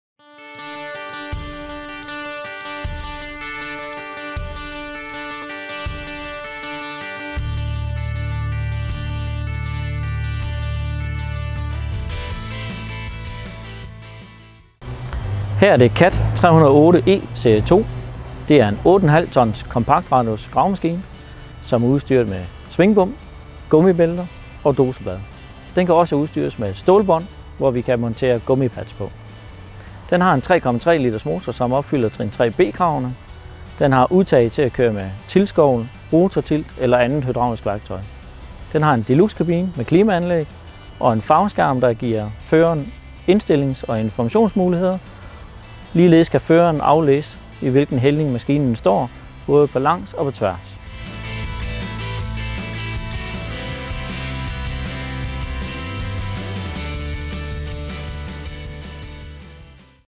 Pon Equipment - Cat 308 E2. 8,5 tons kompakt gravemaskine
Udstyret med svingbom, gummibælter og dozerblad. 3,3 Liters motor, der opfylder trin 3B krav. Deluxe kabine med klimanlæg og farvemonitor.